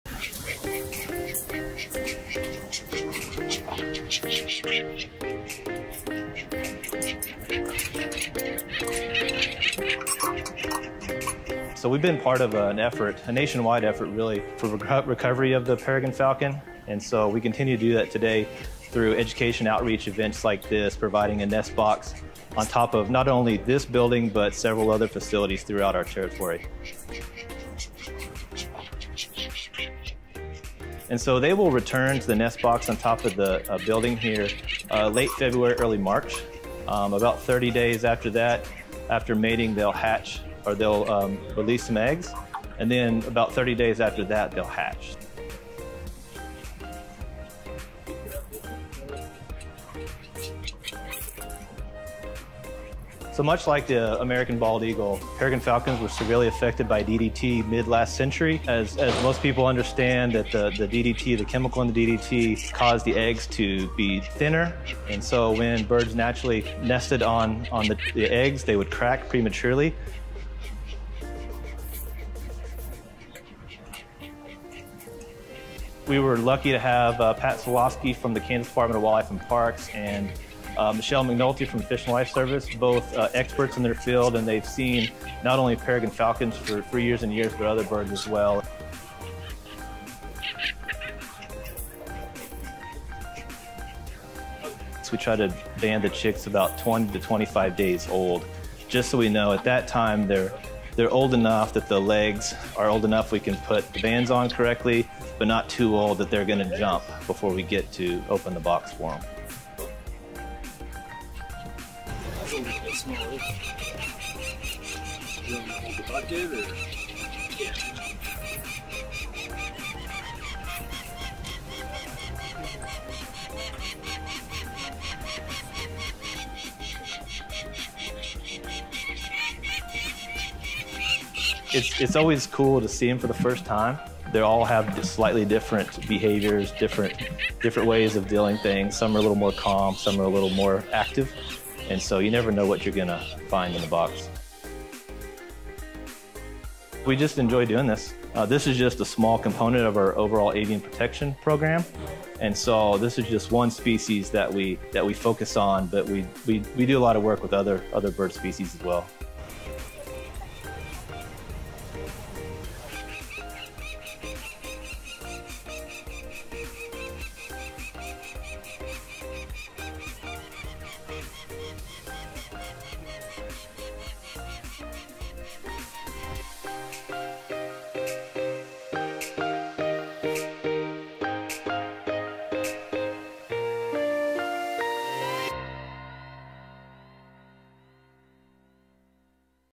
Evergy Bands Falcons at Topeka General Office Building
We banded the young falcons that are nesting on top of our Topeka General Office recently. Our wildlife biologist Eric talks in this video about the process and why Evergy, though our avian protection program, has been involved in helping grow the population of peregrine falcons. 💙💚

Peregrine falcons are the fastest living animals, and you can watch our birds grow quickly before they leave the nest, on our falcon live cams 📹